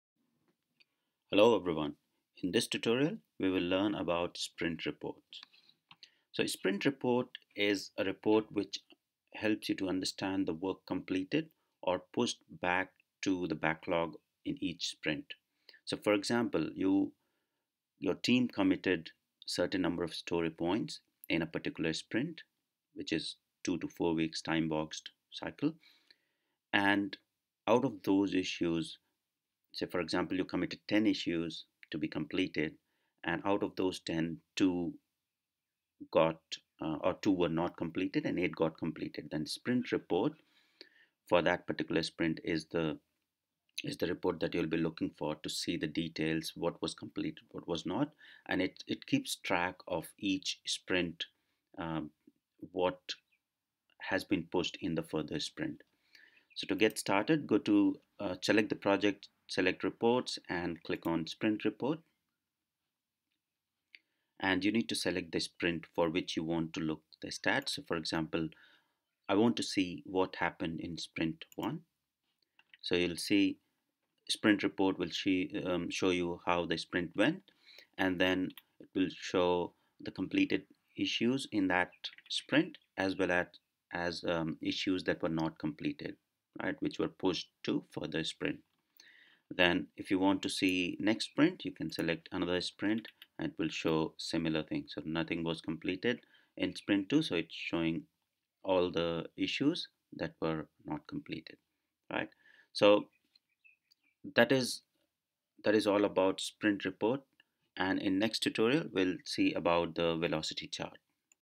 JIRA Tutorial #38 - JIRA Sprint Report | Sprint report in JIRA
In this JIRA tutorial we will learn about JIRA Sprint report and how can you use Sprint report in jira to track the progress of Sprints.